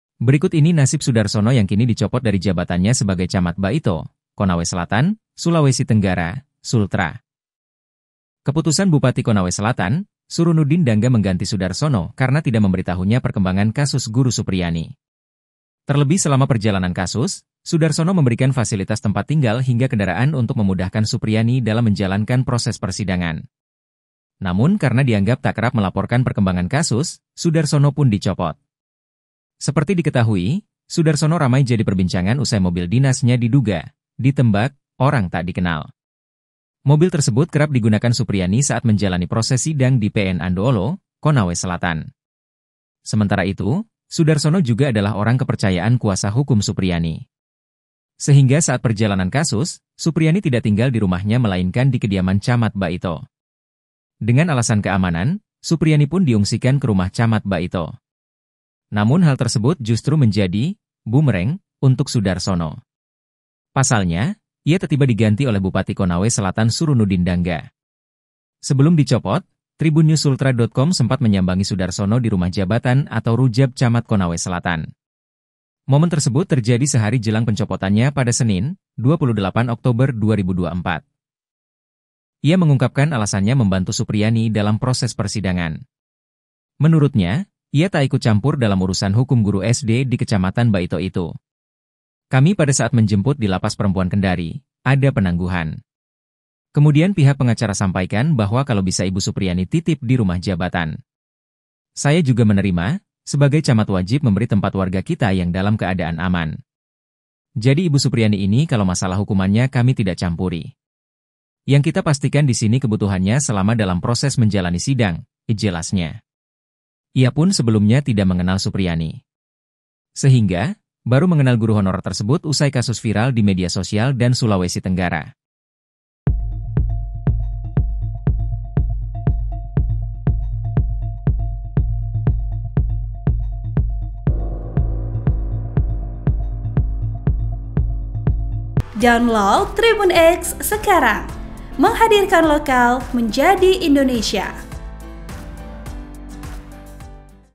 NASIB CAMAT BAITO Sudarsono Malah Dicopot Bupati, Diangggap Bantu Kasus Guru Supriyani
TRIBUN-VIDEO.COM - Berikut ini nasib Sudarsono yang kini dicopot dari jabatannya sebagai Camat Baito, Konawe Selatan, Sulawesi Tenggara (Sultra).

Keputusan Bupati Konawe Selatan, Surunuddin Dangga mengganti Sudarsono karena tidak memberi tahunya perkembangan kasus guru Supriyani.